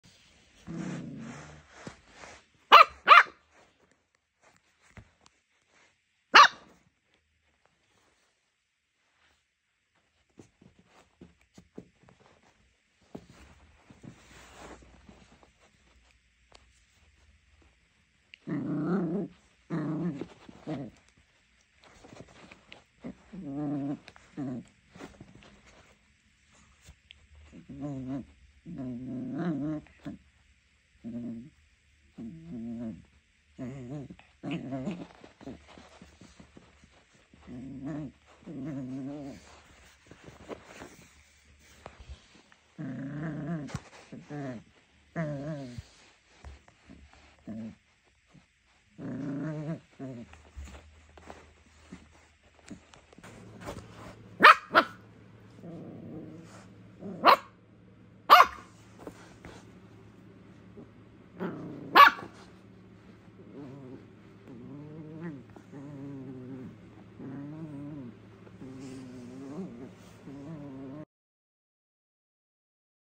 Jack russell 3 month old playing with shocks tug war
Junior is a jack russell terrier and have a very active playing nature. Cute and lovely good boy. He is in teething stage and love to bite anything they see.  Hiding under the bed🐶